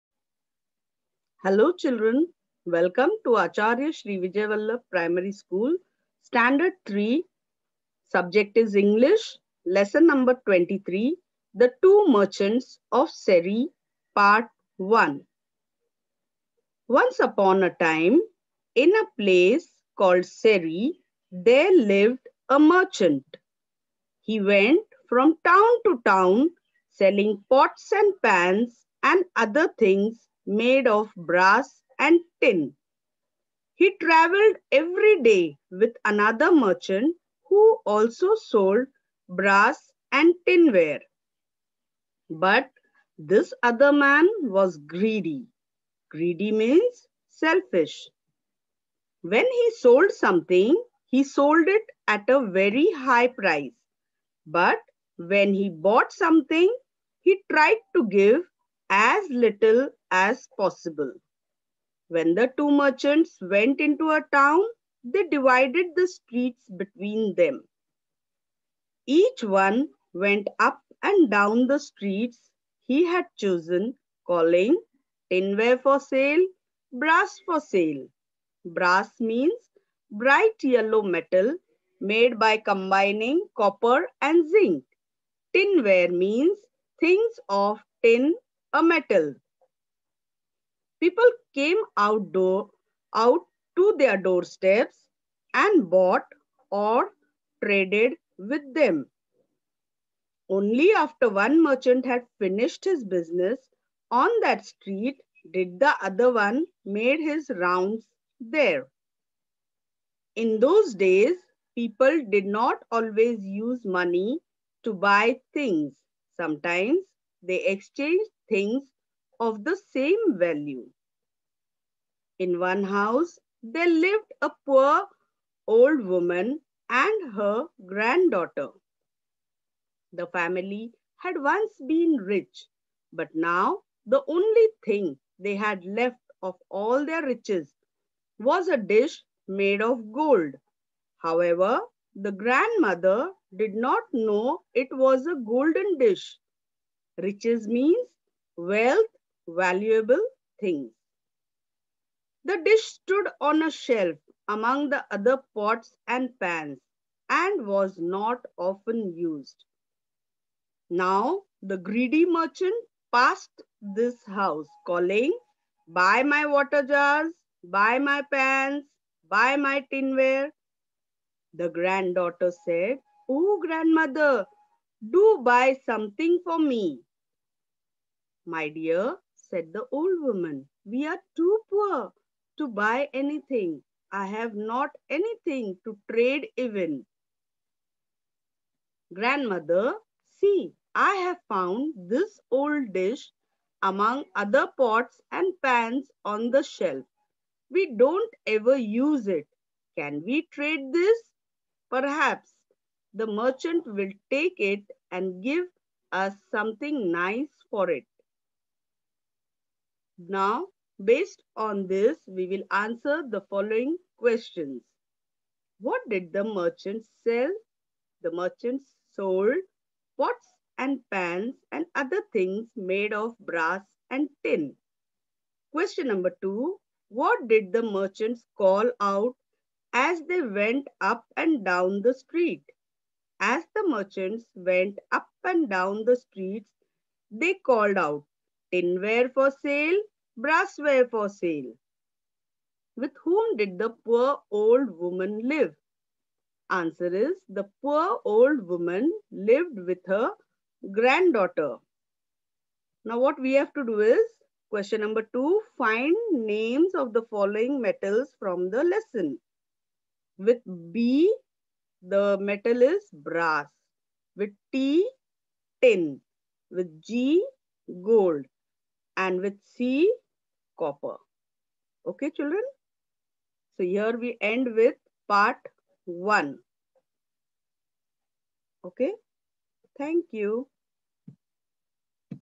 Vijay Vallabh Schools_Grade III_English_The two merchants of Seri
Teacher's Name: Mrs. Farida Sayyad
Grade : III
Subject: English
Topic: The two merchants of Seri